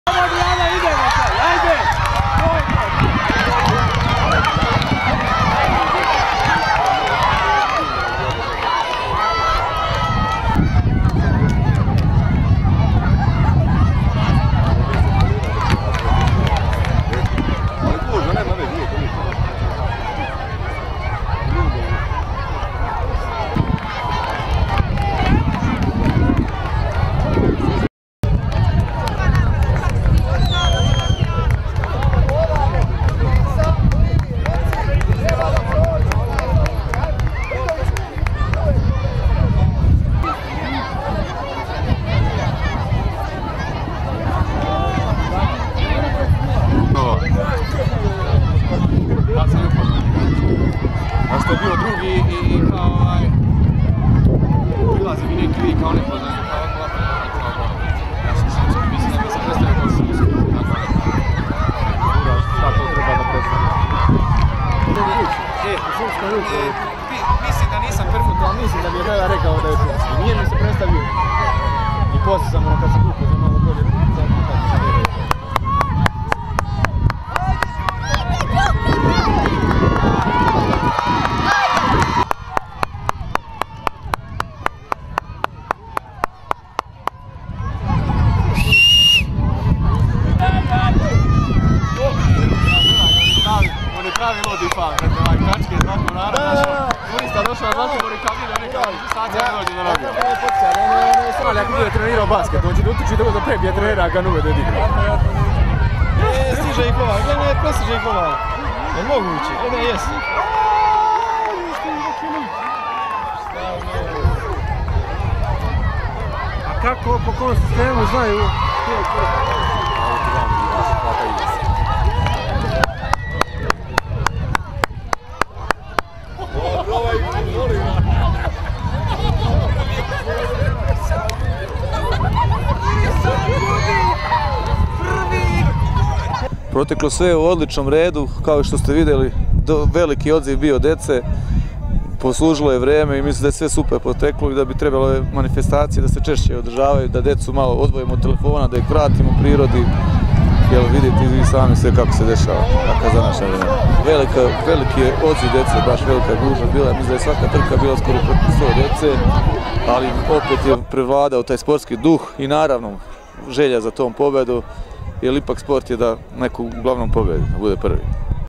Крос РТСа на Златибору
Традиционални крос Радио телевизије Србије одржан је на стадиону Швајцарија уз учешће око 700 деце из основних школа „Димитрије Туцовић“, „Миливоје Боровић“, „Саво Јовановић Сирогојно“  и предшколске установе „Радост“ у организацији Спортског центра Чајетина, Спортског савеза Чајетина и клубова са територије општине.